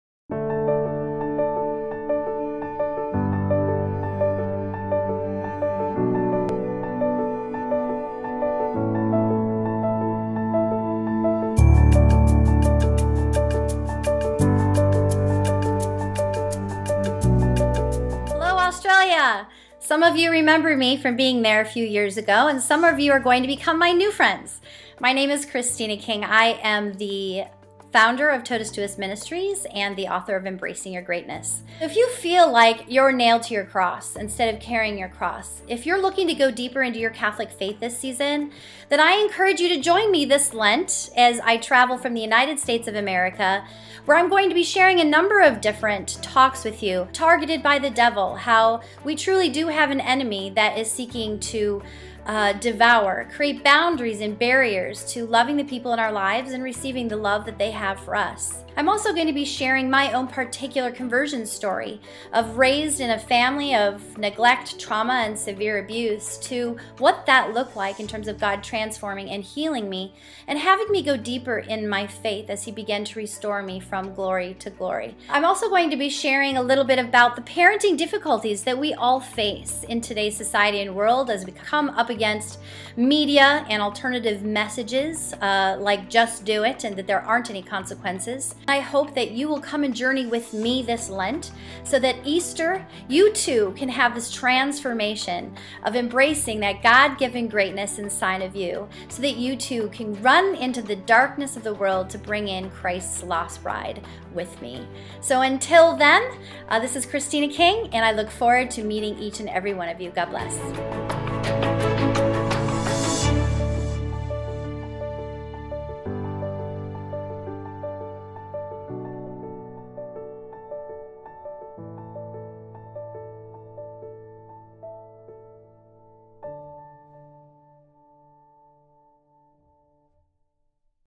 Christina King 2018 Embracing Your Greatness Tour
Christina King is in Sydney and Melbourne from February 16 to 25. Christina is an author, freelance writer, national radio show host with Radio Maria and an international speaker with a highly dynamic and interactive style with over 17 years of experience inspiring audiences to embrace the beauty of the Catholic faith and God's plan for their life. For the full schedule or her talks in Sydney and Melbourne please